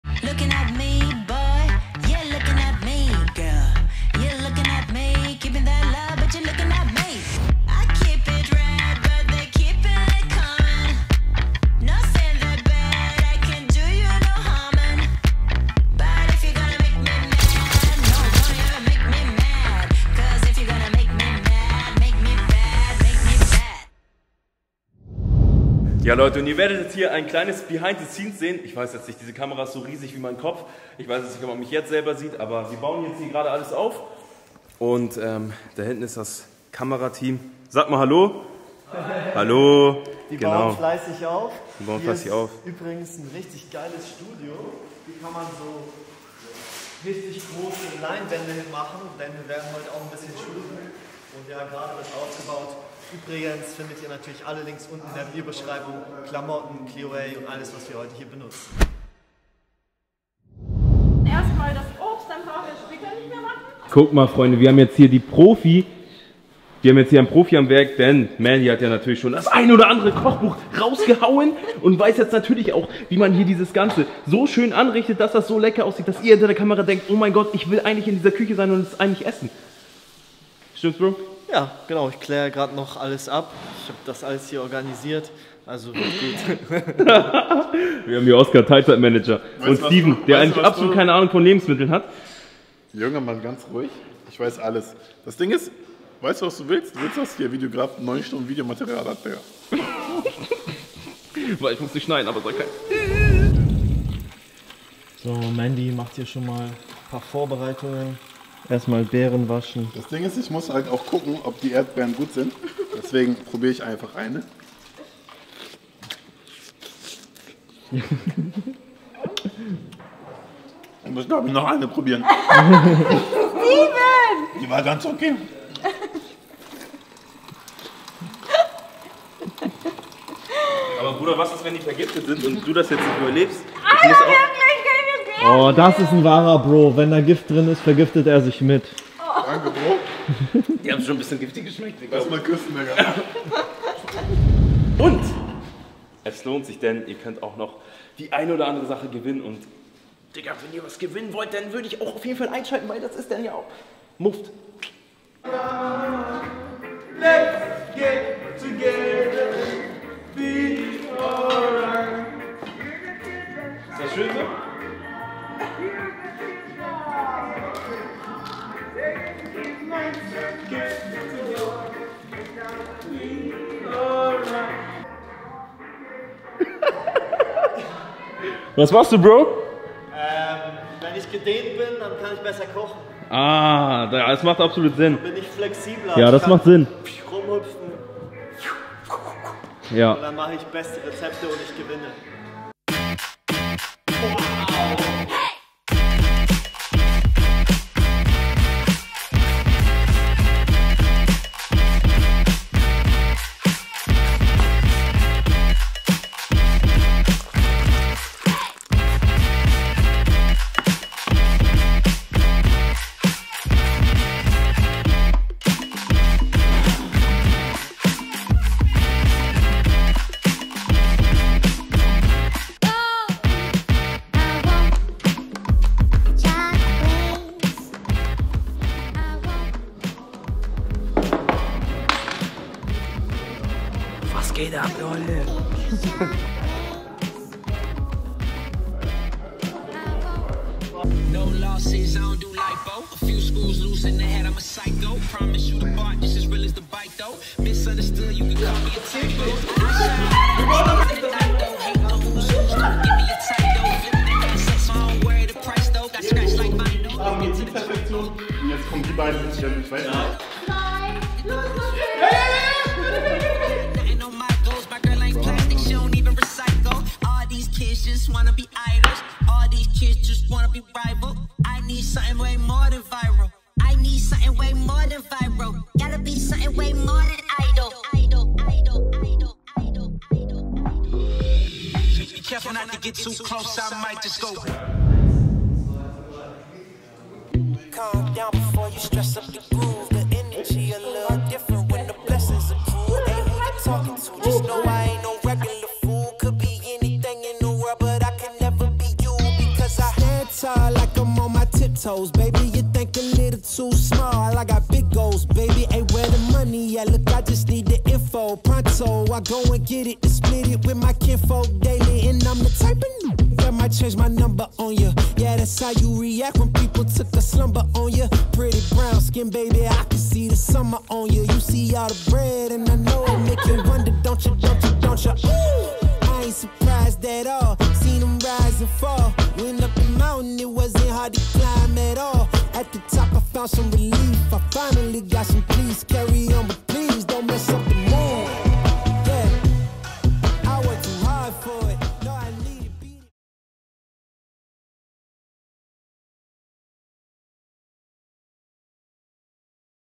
LIVE | Hinter den Kulissen mit FitnessOskar, Healthy_Mandy, Wing_Steven und Wing_Dodo
Hallo liebe Myprotein Familie! Im heutigen Video erfahrt ihr, wie es hinter den Kulissen bei einem MyProtein-Live-Event abläuft. Schaut zu wie Oskar, Mandy, Dodo und Steve sich auf das Event vorbereiten und wie das Fotoshooting gelaufen ist. Viel Spaß beim zusehen!

Wenn Dir das Video gefällt und Du Myprotein Fan bist, vergiss nicht diesem Video ein "👍" zu geben!

00:00 Einführung
01:00 15 Minuten vor dem Live Event
01:45 Die Vorbereitungen
03:13 Das Live Event
04:56 Fotoshooting

Werde Teil der deutschen Myprotein Community!